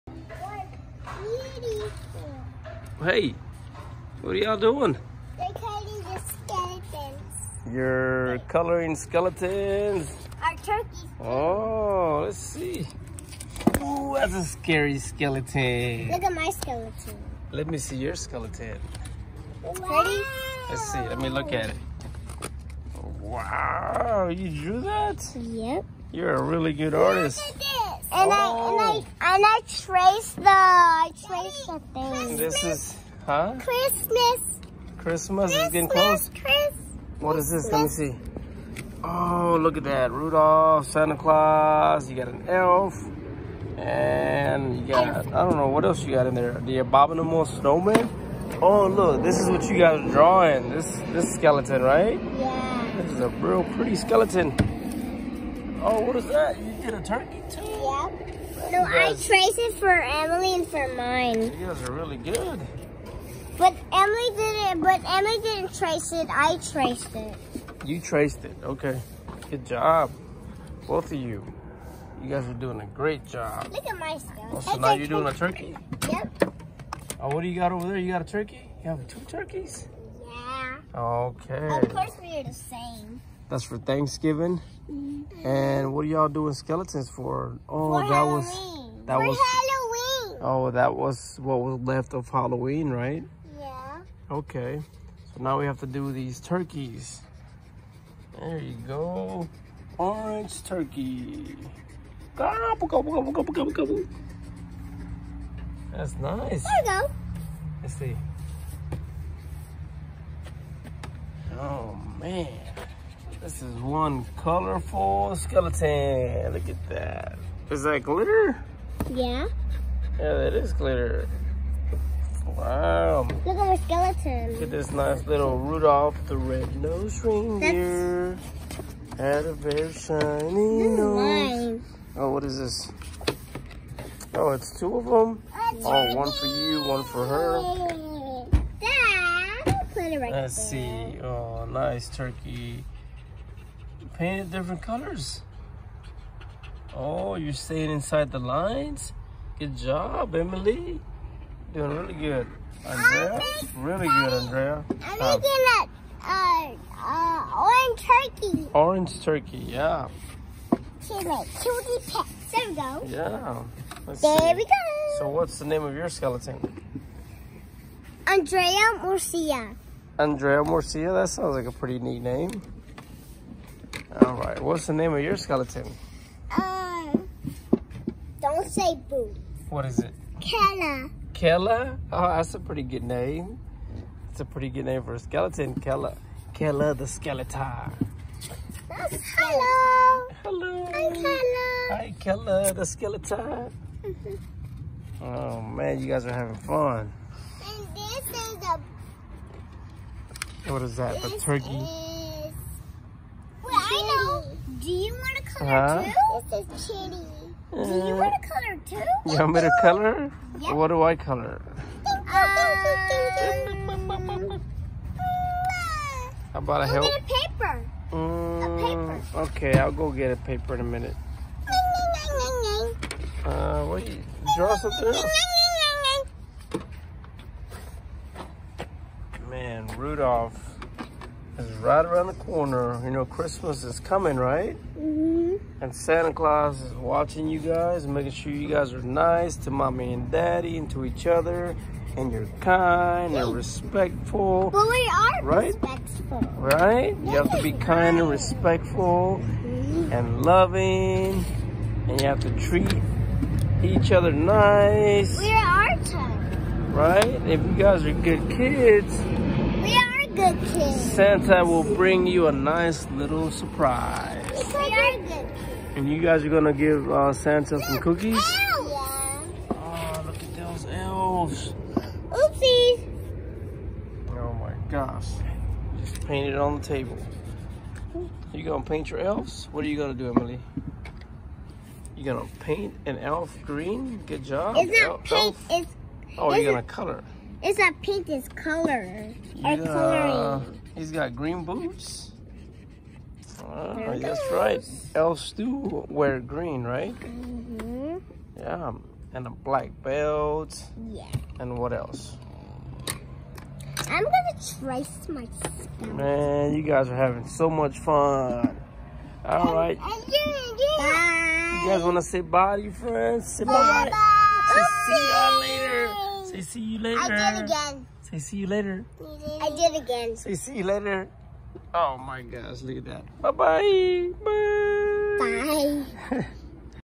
Early Christmas Spirit 🎅 and drawing a Skeleton
Andre and Emily start the Christmas spirit early and draw a turkey. Fun family video